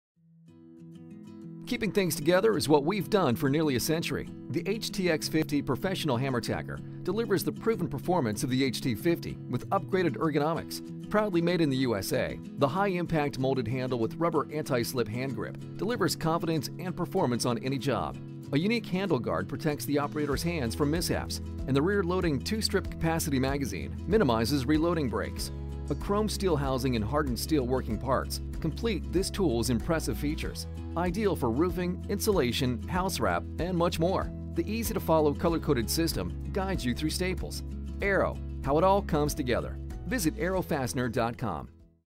Arrow's Professional Hammer Tacker
The HTX50 Professional Hammer Tacker from Arrow Fastener delivers the proven performance of the HT50 with upgraded ergonomics. The high impact molded handle with rubber anti-slip hand grip delivers confidence and performance on any job. A unique handle guard protects the operator’s hand from mishaps and the rear loading, two strip capacity magazine minimizes reloading breaks. A chromed steel housing and hardened steel working parts complete this tool’s impressive features. Proudly made in the USA, this tool is ideal for insulation, house wrap, roofing underlayment and flooring.